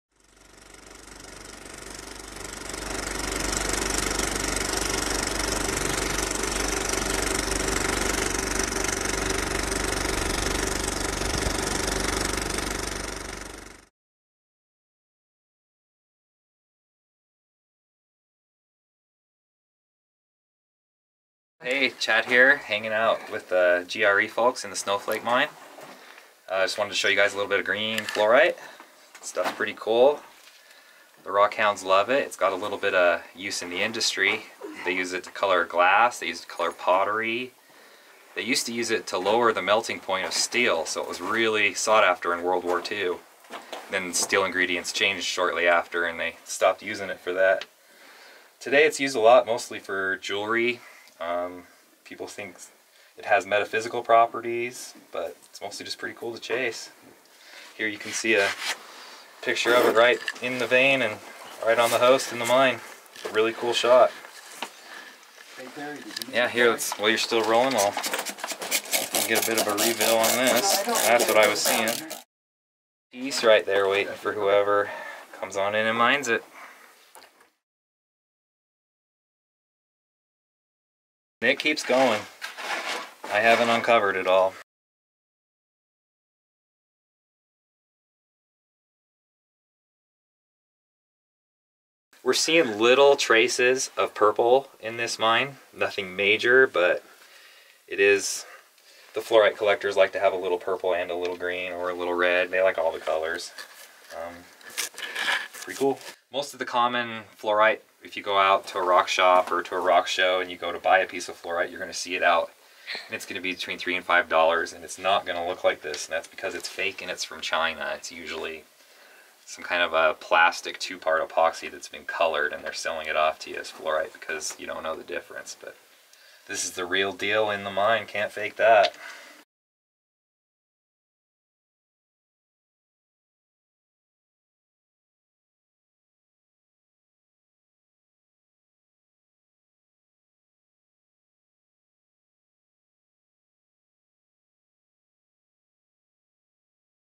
Snowflake Mine, Colorado, Gold Rush Expeditions, 2014
Highlighting some green Fluorite in the Snowflake Mine. Fluorite is sought after by rock-hounds, and it also has some commercial uses including coloring glass and pottery. It was also used to lower the melting point of steel back in the 1940's.